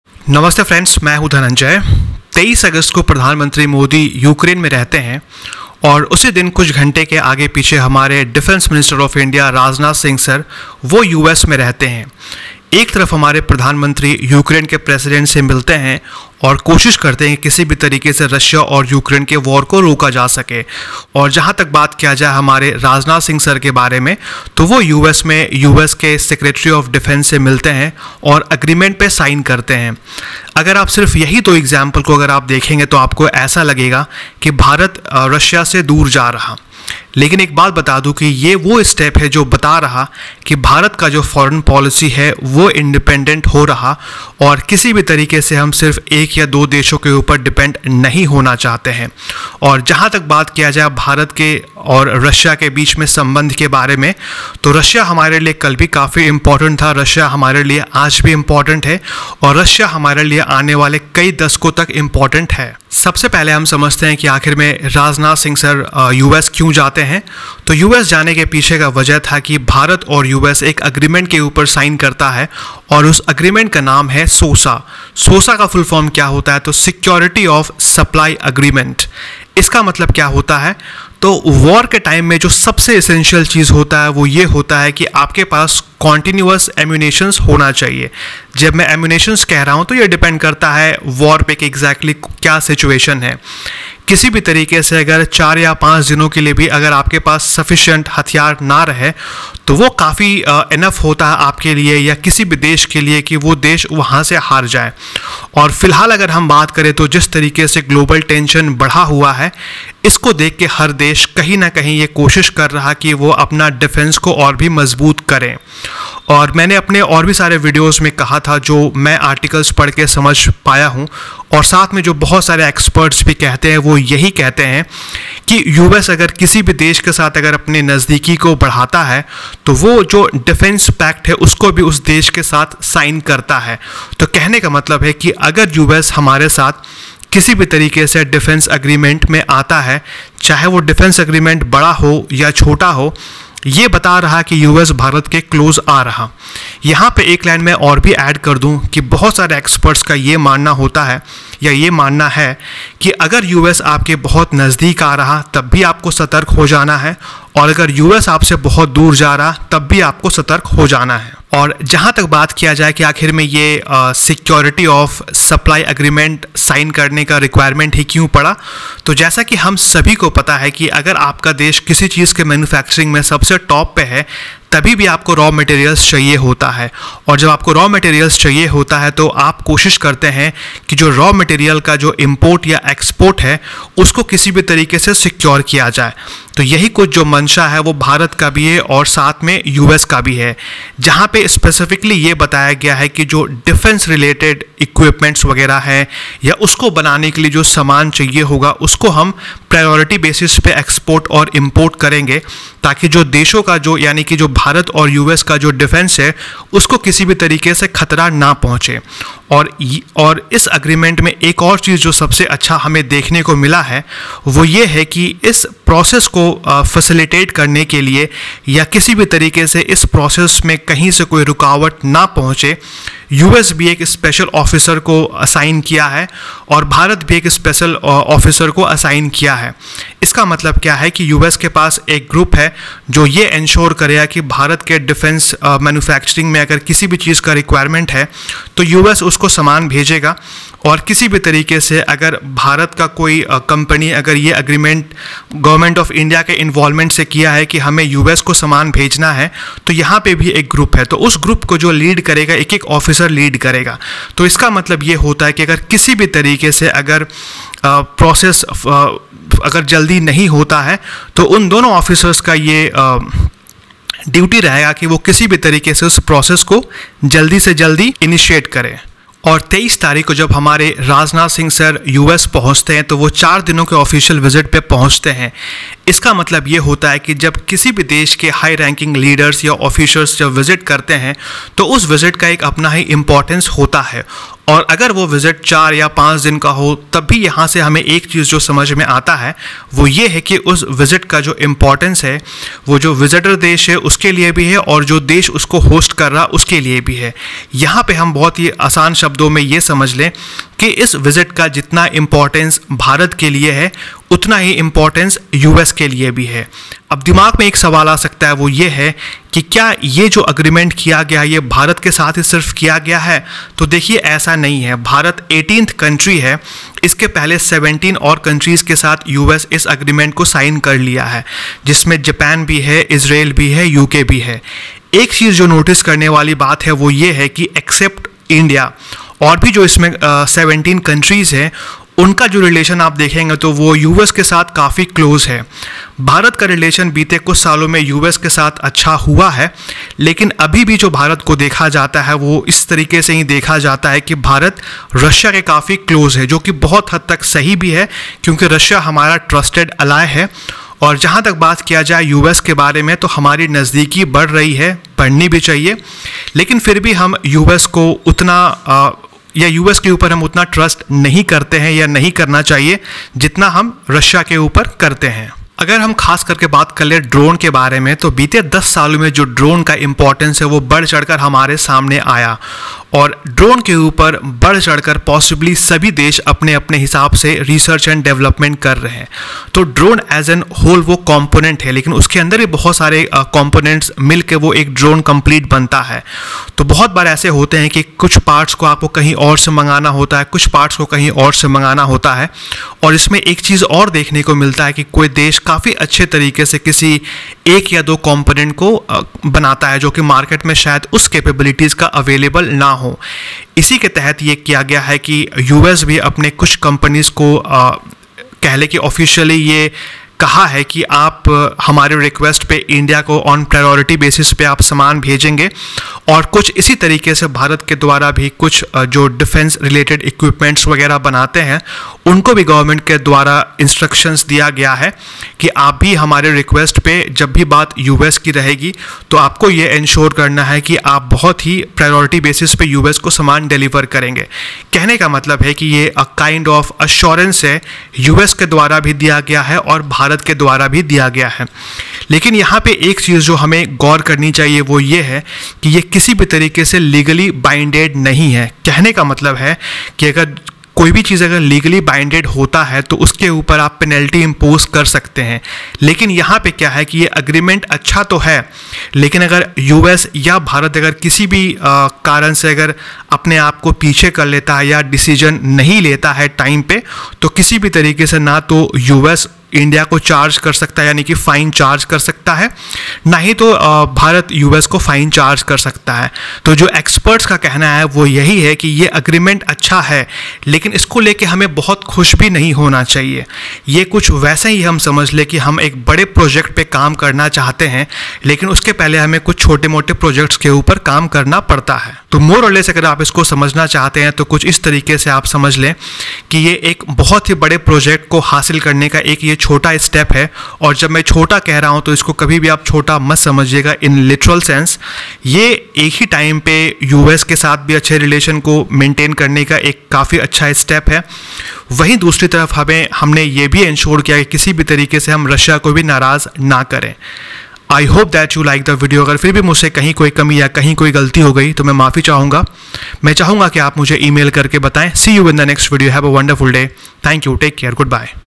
India-USA Sign key Defence Pact
India and the US signed two important agreements on Thursday, one of which was a Memorandum of Agreement for Assignment of Liaison Officers and the other being a non-binding Security of Supply Arrangement (SOSA), as Defense Minister Rajnath Singh began a four-day official visit to Washington, DC.

The US and India will be required by the SOSA to give each other priority assistance for products and services that advance national defense.

According to a US Department of Defense release, "the arrangement will enable both countries to acquire the industrial resources they need from one another to resolve unanticipated supply chain disruptions to meet national security needs."